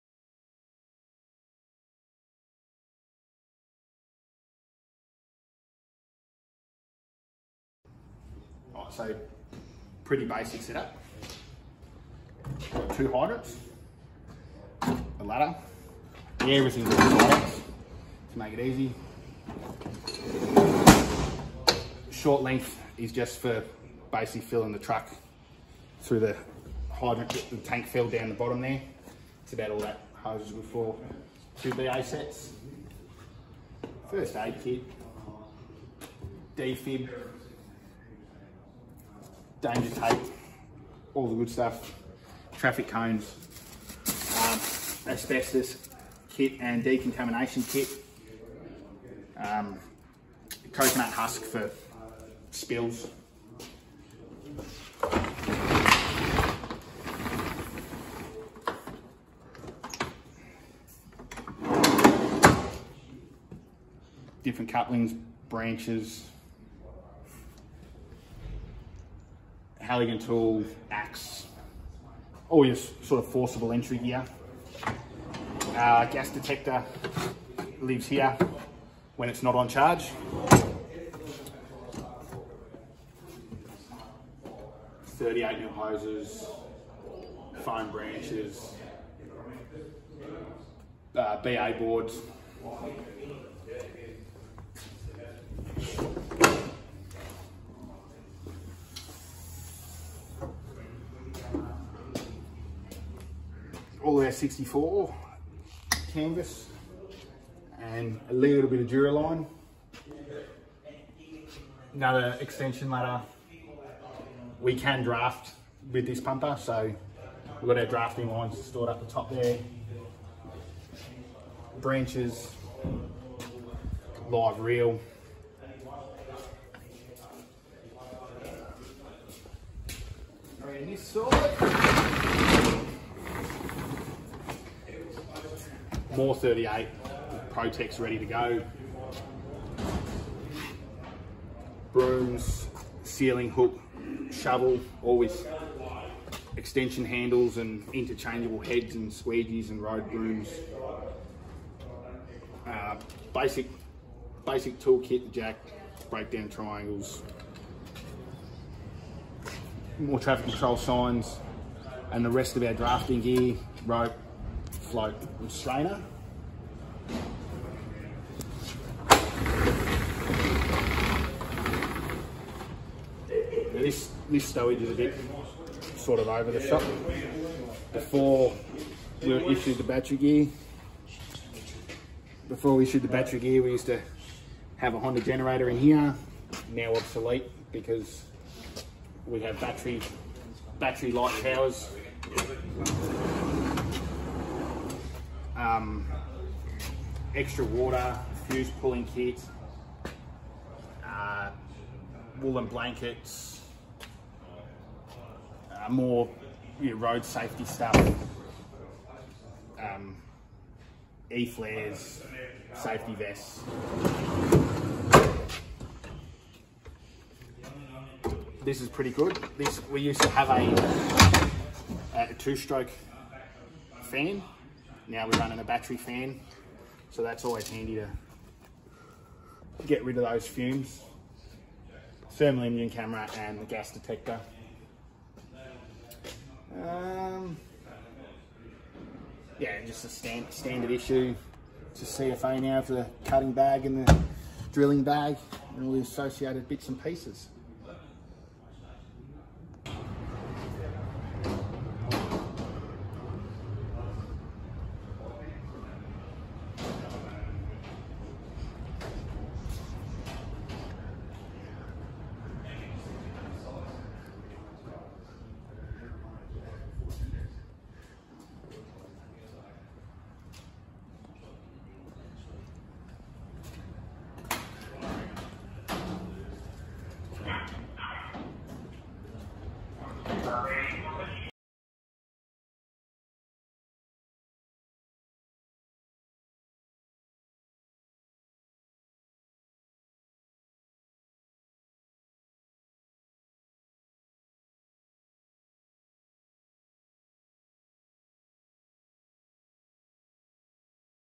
Inside CFA: episode 3 | Winchelsea CFA
From light demos, to a tour of their light pumper thanks to the volunteers at Winchelsea Fire Brigade